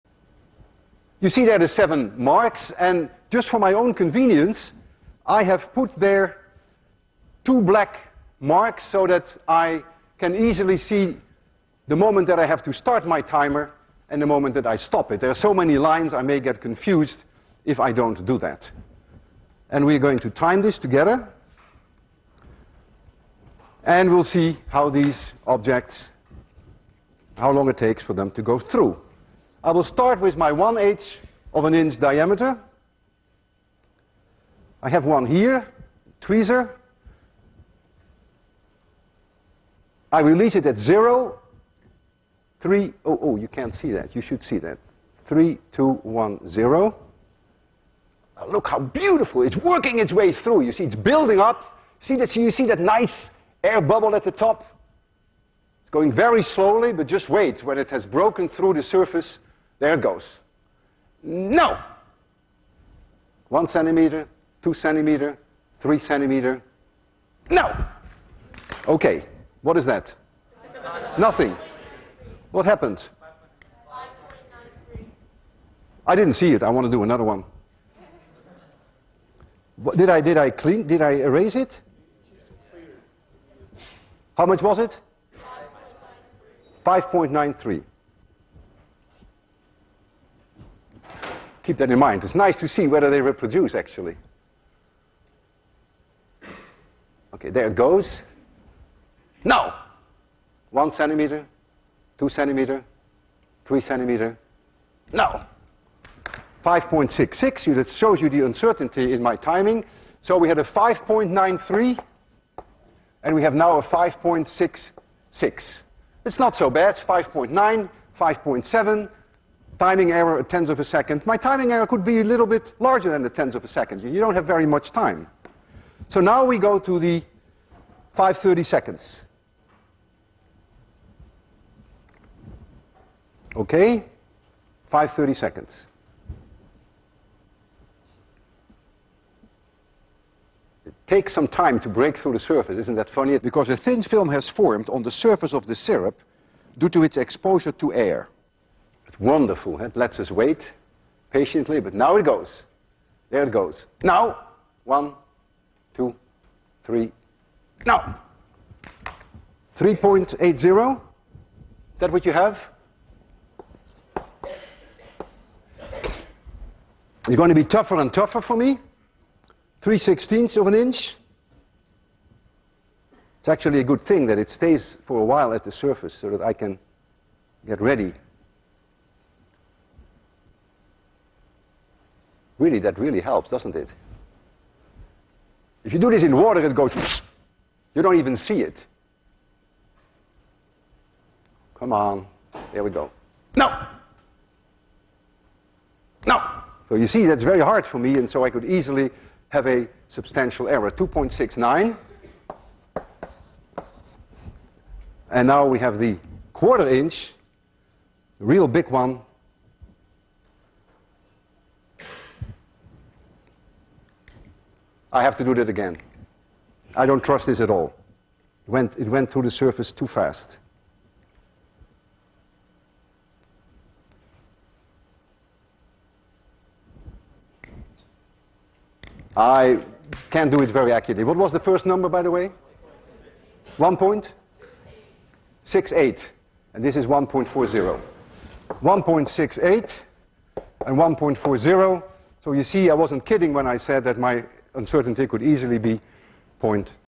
WLviscous.avi
In his lecture on viscosity ), Walter Lewin measures the terminal velocity of various metal bearing balls of different diameters, but of the same density, falling vertically in a container filled with Karo light corn syrup.